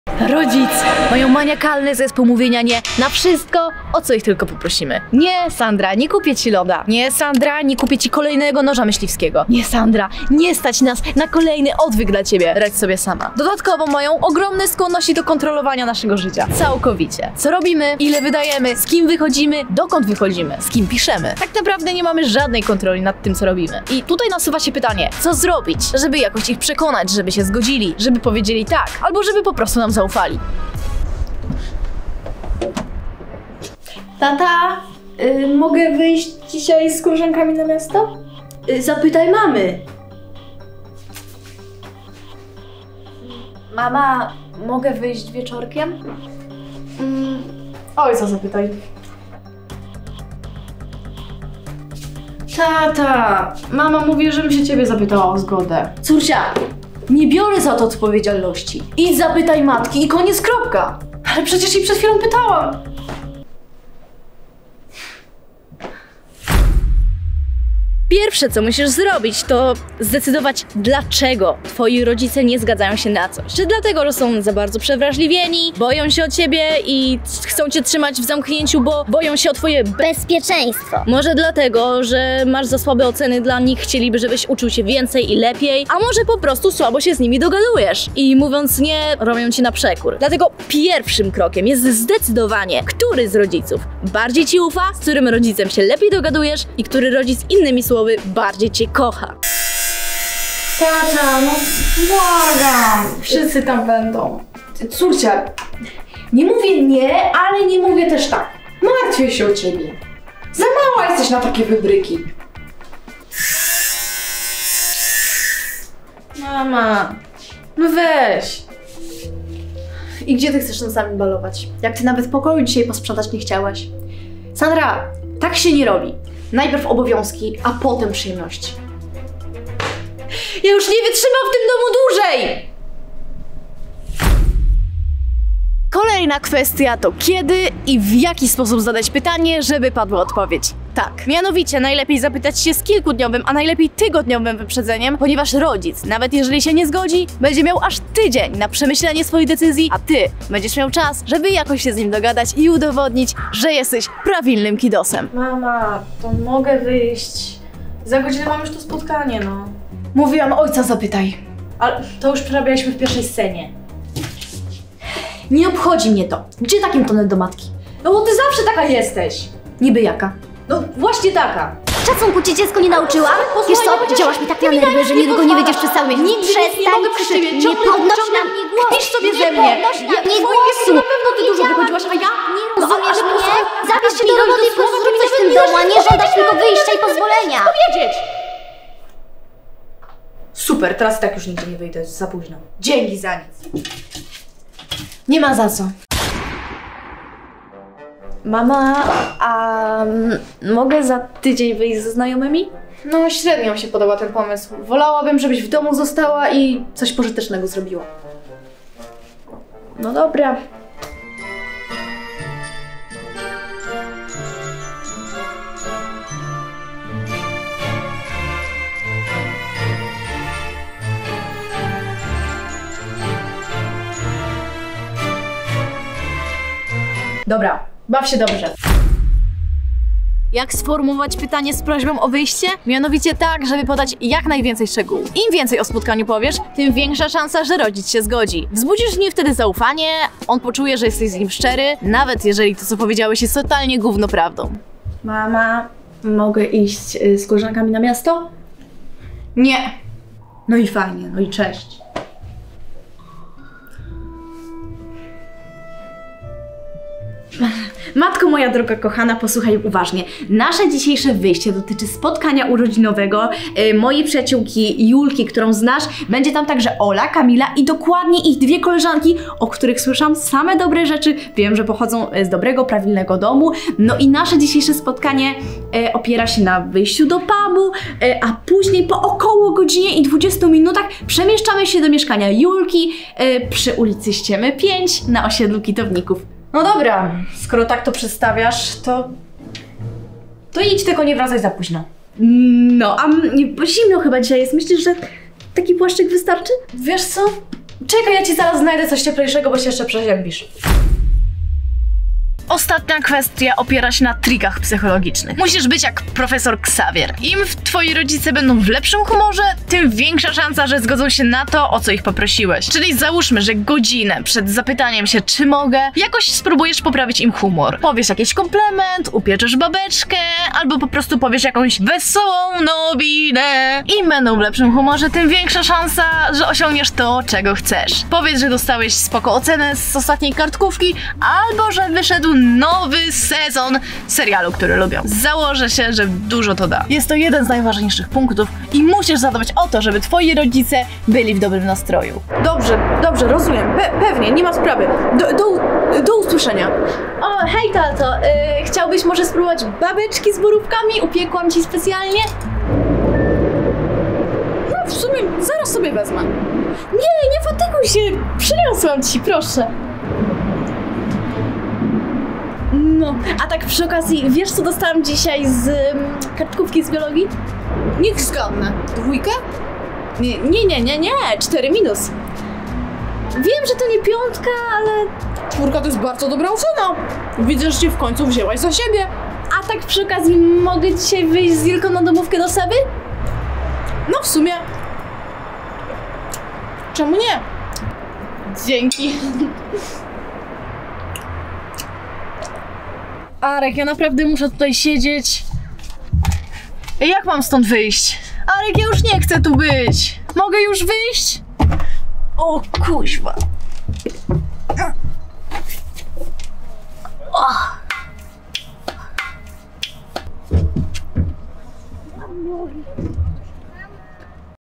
jak sprawić żeby rodzice się na wszystko zgadzali
Rodzice mają maniakalny zespół mówienia „nie” na wszystko o co ich poprosimy...

zostaw suba